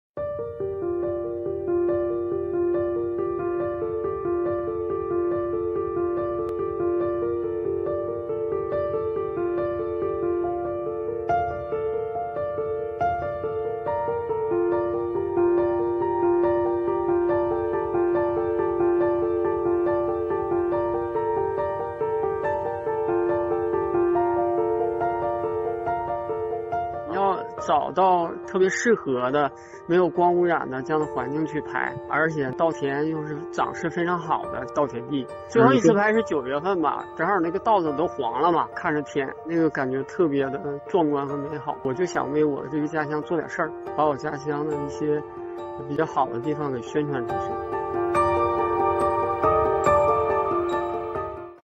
太治愈了！延时记录稻田与星空的奇妙组合
9月21日，一段延时记录星空和稻田的视频获得关注。视频中星空璀璨，稻田由翠绿变成金黄，有网友评价画面太美好像在童话里。22日，视频拍摄者曲玉宝告诉新京报，整个视频拍摄历时5个月，地点位于黑龙江富锦市。曲玉宝表示，希望用延时星空视频拍摄记录家乡的美好，让更多的人了解他的家乡。

It's so healing! Time-lapse recording of the wonderful combination of rice fields and starry sky. The video shooter Qu Yubao told The Beijing News that the entire video was filmed for 5 months and was located in Fujin City, Heilongjiang.❤️